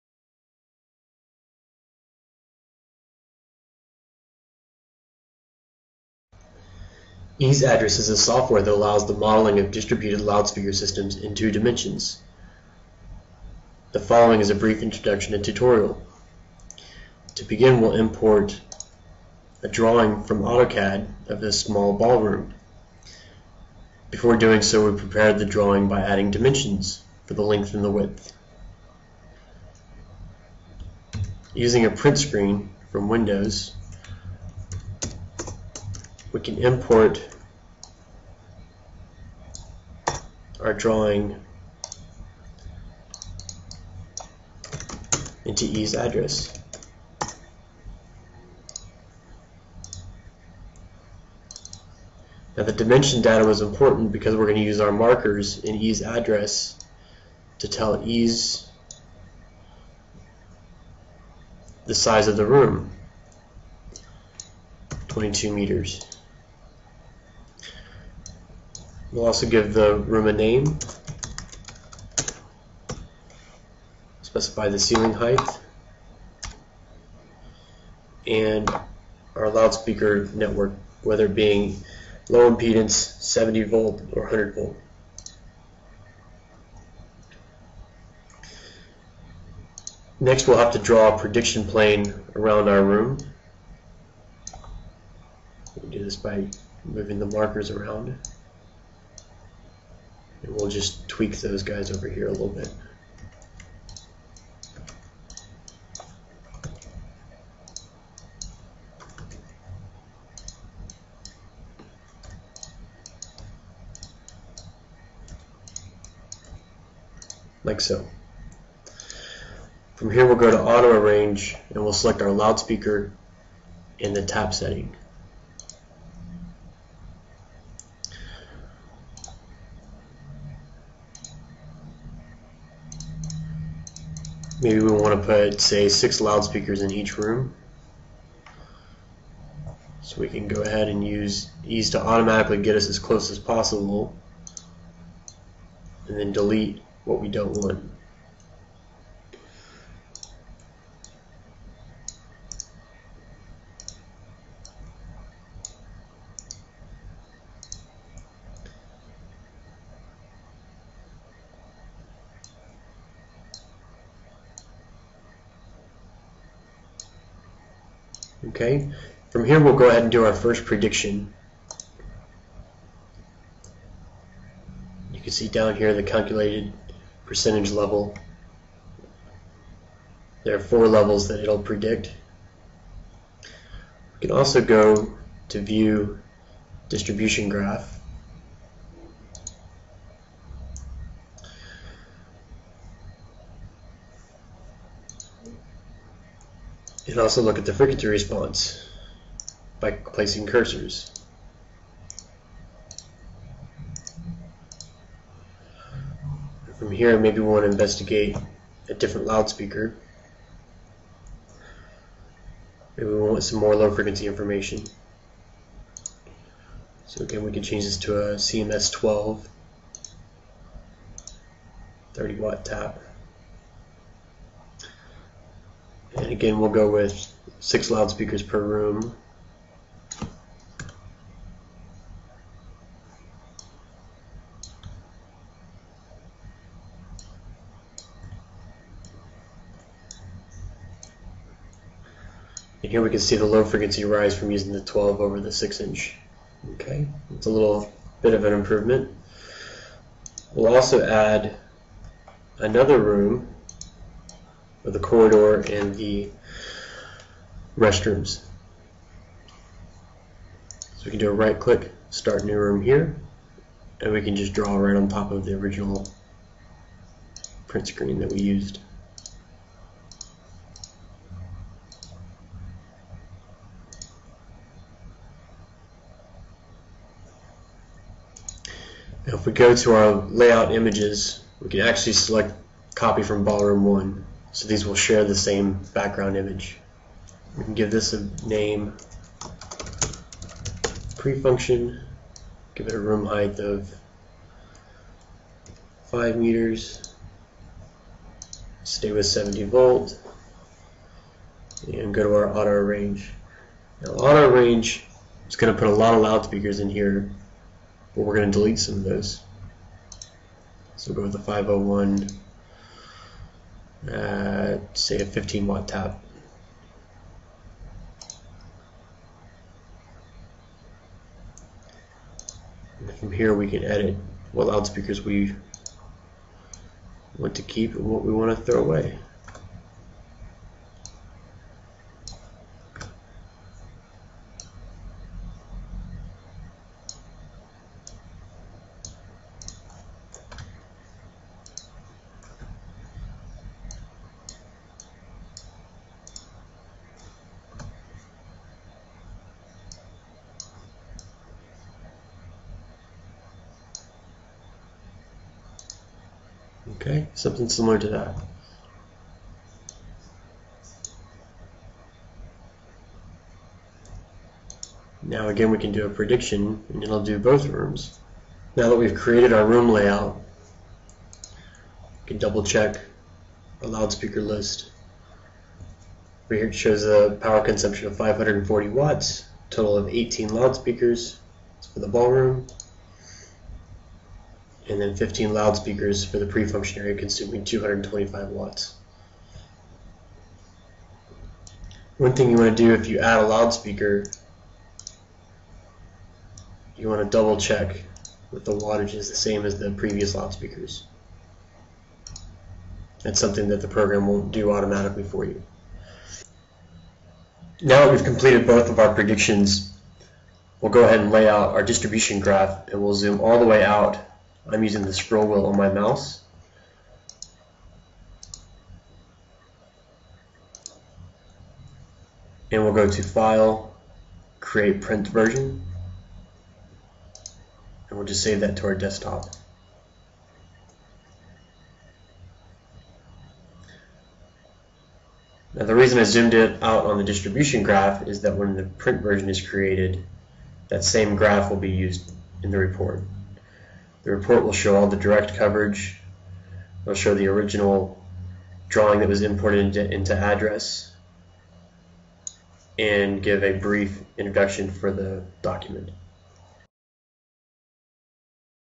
Tannoy EASE Address Tutorial.mov
EASE Address is the unique design verification software package that enables system designers to accurately position, model and confirm ceiling distributed speaker systems utilising Tannoy loudspeaker products. Here we take you through a tutorial of best practice with the software, demonstrating how powerful but easy to use EASE Address is - an indispensable tool for the audio system designer.